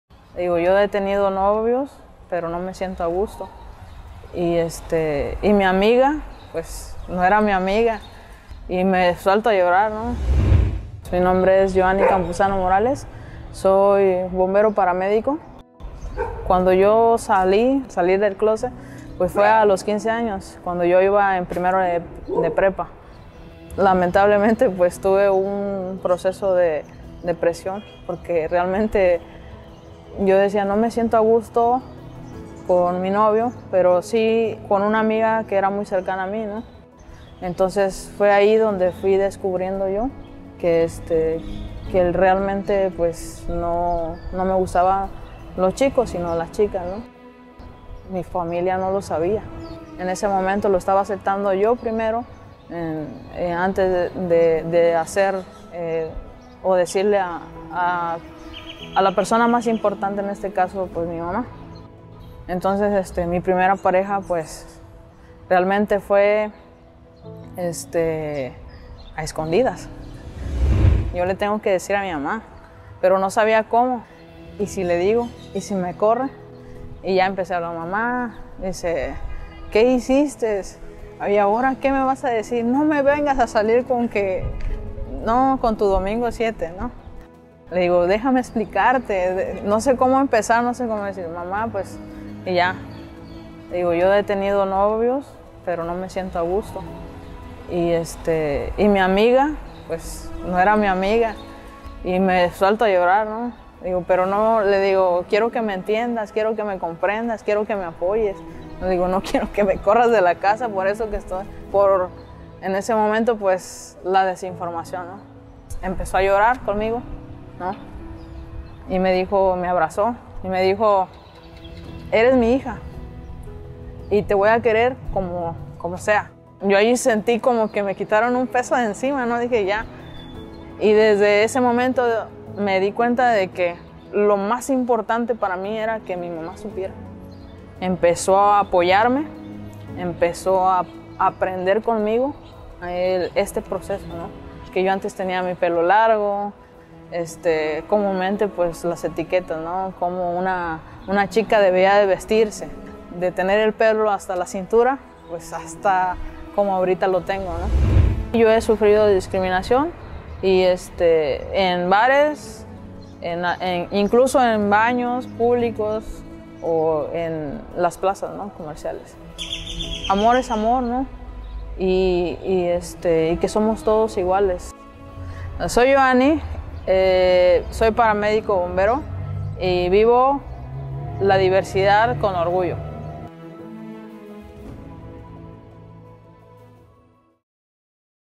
DiversidadConOrgullo 🏳️‍🌈 | Jhoany Campuzano, bombera paramédica
Muchas veces la desinformación puede llevarnos a ocultar de las personas que más amamos quienes realmente somos.

Conoce a Jhoany, egresada de la Licenciatura en Protección Civil y Emergencias quien con valentía enfrentó sus miedos para lograr vivir su diversidad con orgullo. 🏳️‍🌈⛑️